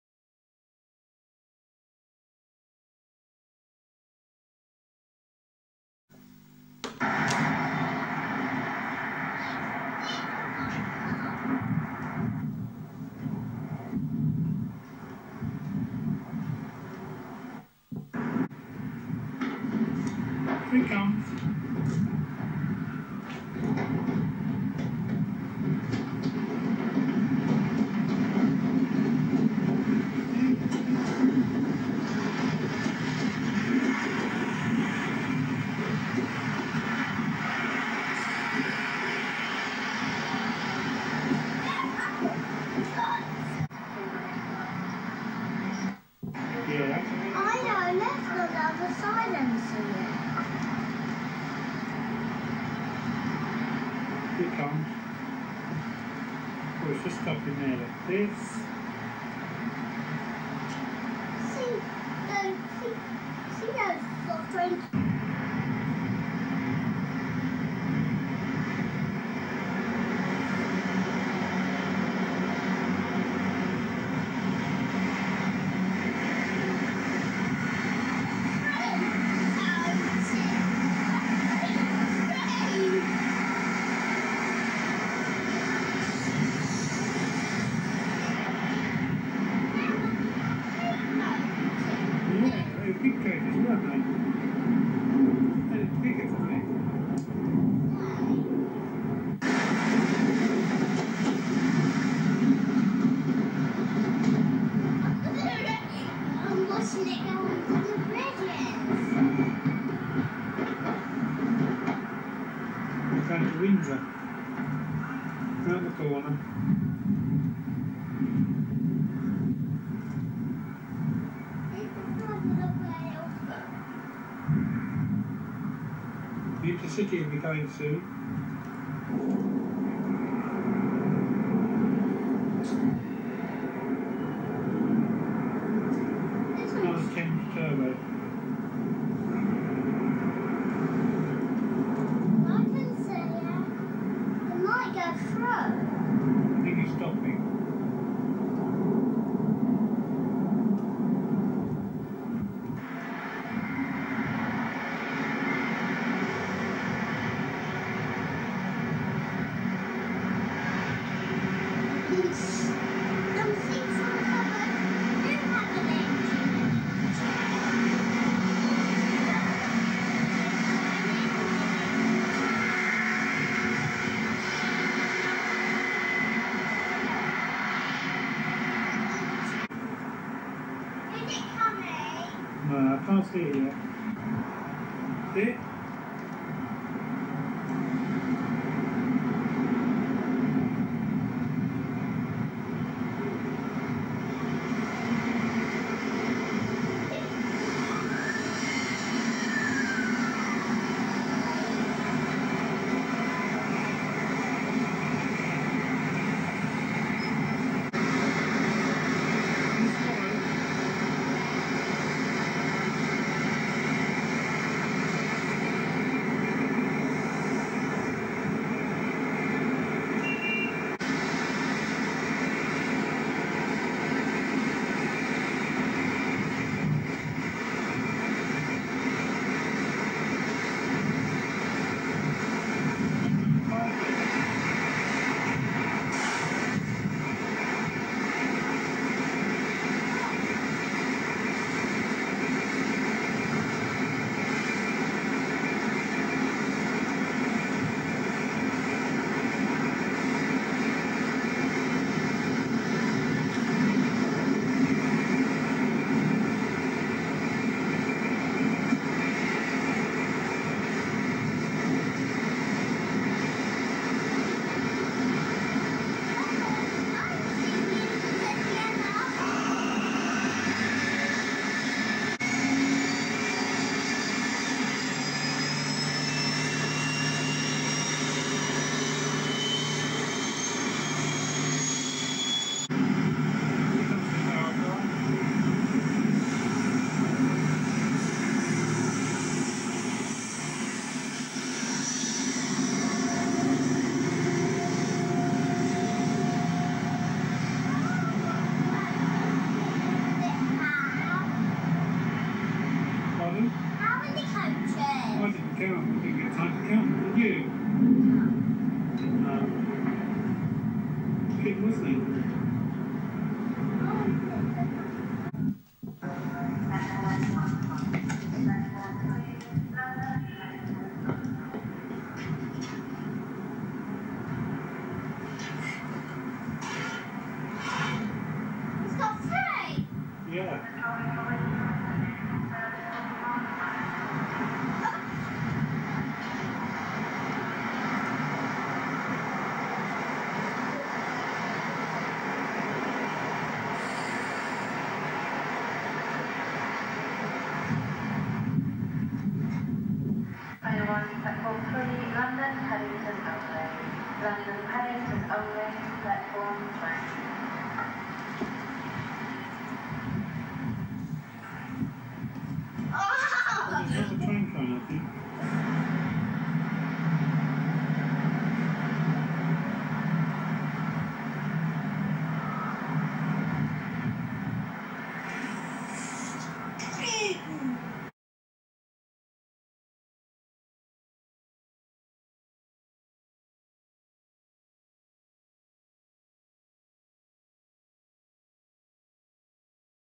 Watching trains at Slough on the 21st February 1993
Henry's Adventures Archives Episode 9.

A short video featuring a few passenger trains at Slough including Class 165 and 166 Thames Turbos and a HST then know as the Intercity 125 calls at the station.

For more pictures and videos follow Henry's Adventures on Facebook and Instagram!